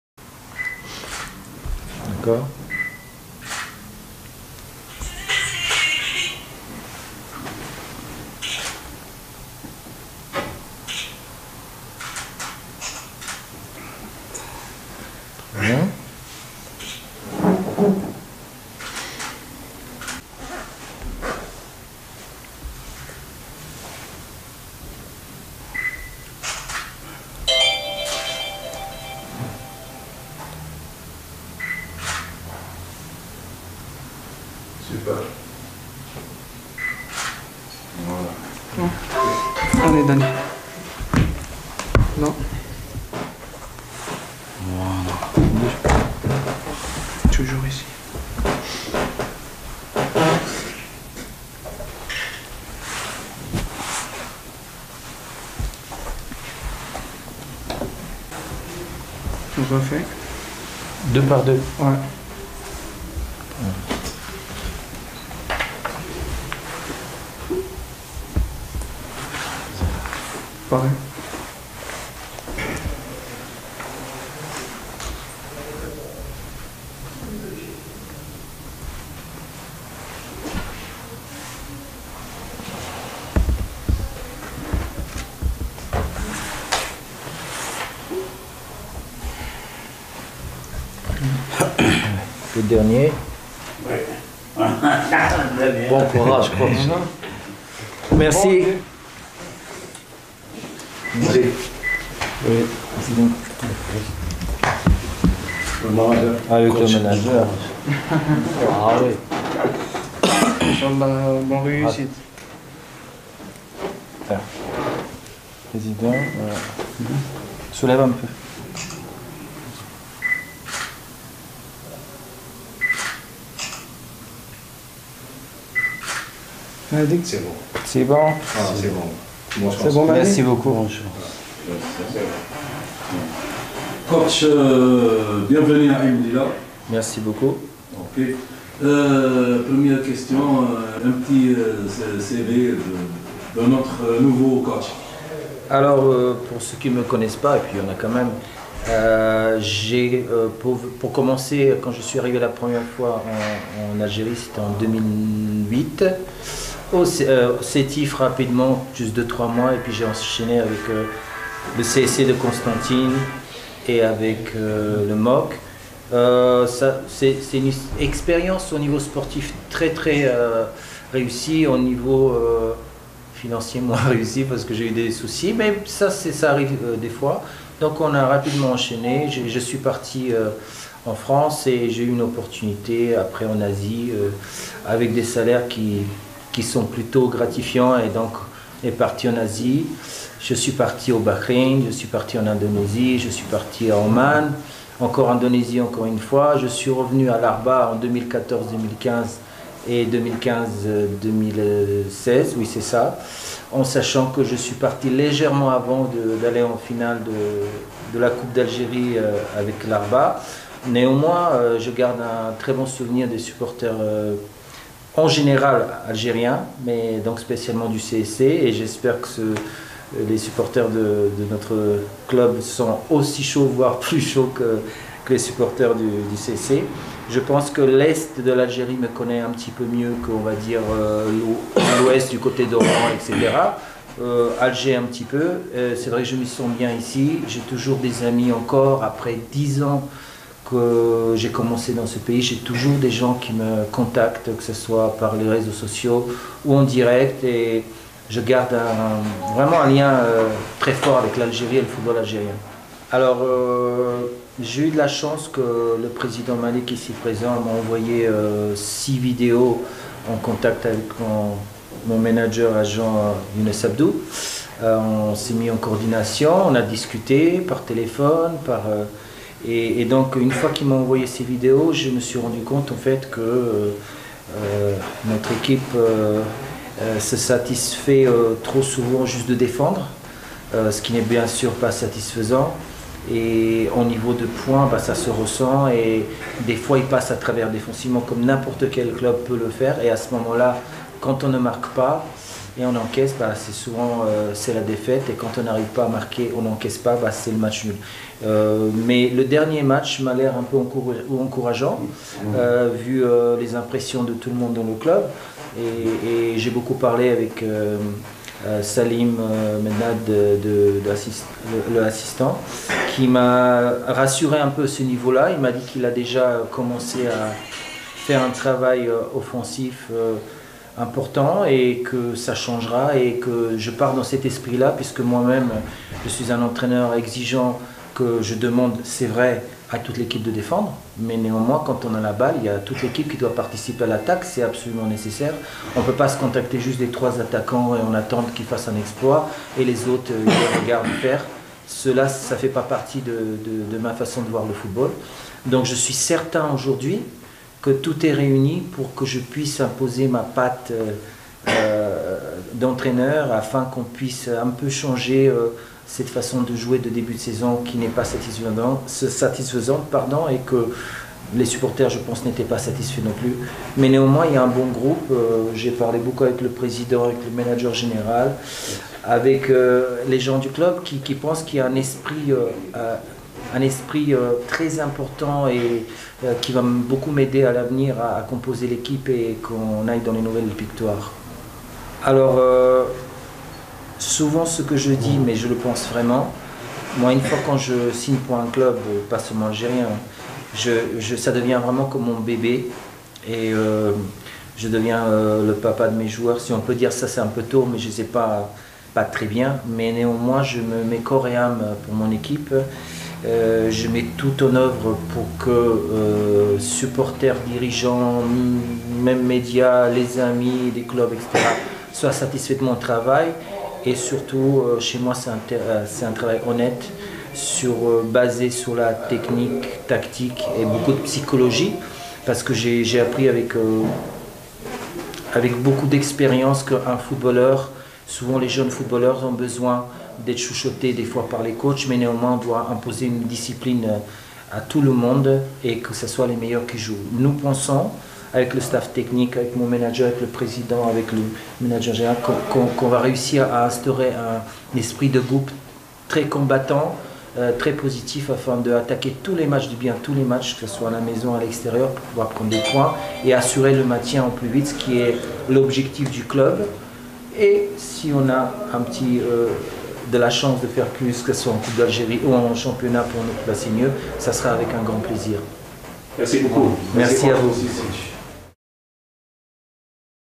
Younes Abdou
Le manager Younes Abdou segnieture de son entreneur Daniel Darko janakovic à ASAM AIN MLILA